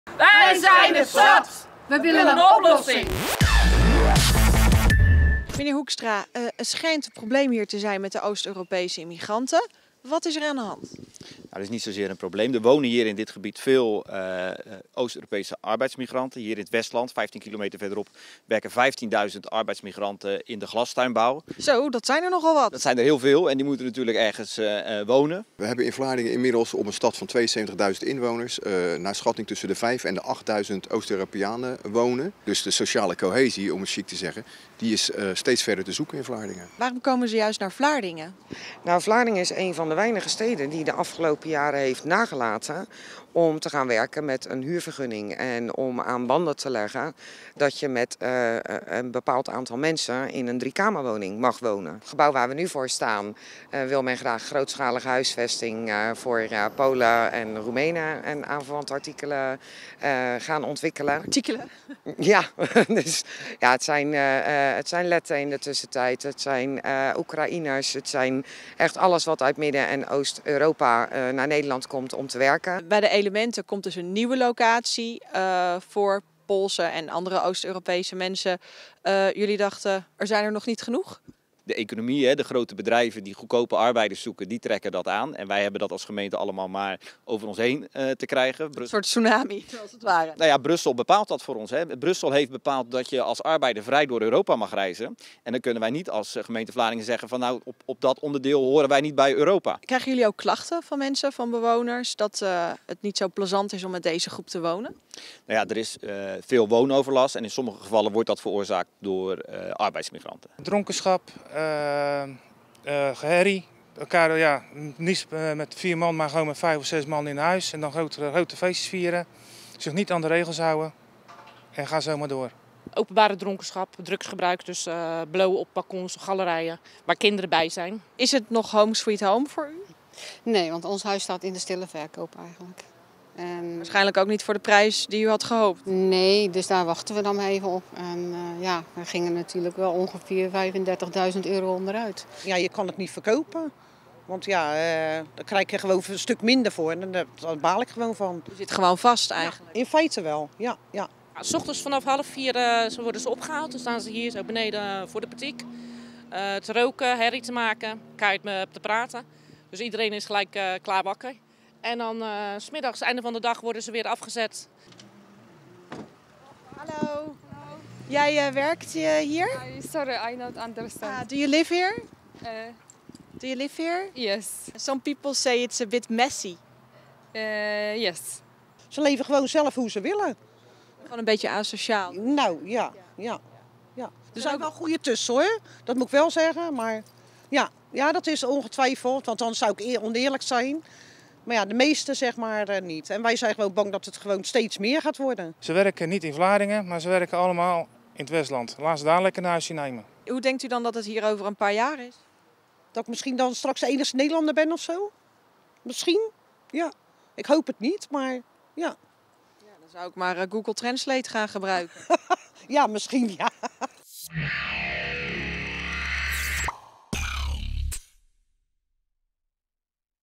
Vlaardingers zijn MOE
Vlaardingen gaat al tijden gebukt onder overlast van Midden en Oost-Europeanen. Buurtbewoners klagen steen en been over onder andere drugs- en drankgebruik, lawaai en pissende buren. Nu er weer een flat wordt omgetoverd voor huisvesting voor zogeheten MOE-landers is de maat vol.